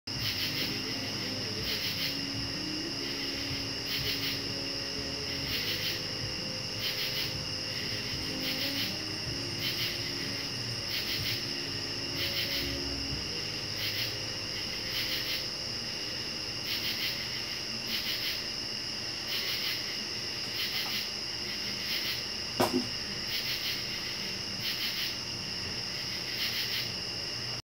Shishkabug
spider wrapping his snack for later